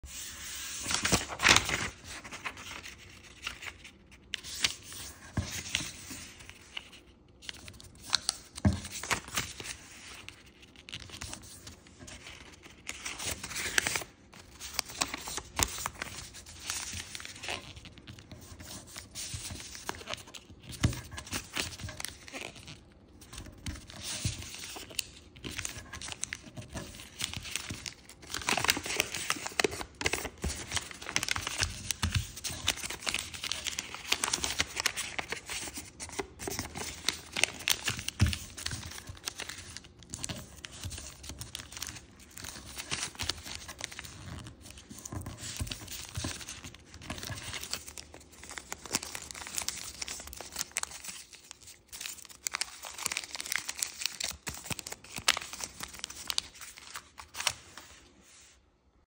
Kimono Patterns Art Asmr Design Paper
Beautiful and elegant Kimono pattern origami paper made into a paper crane. 1 minute shorts Art Asmr video making Asmr sounds and Asmr triggets from paper folding. Relaxing Asmr paper crinkles are definitely made from the kimono pattern origami paper.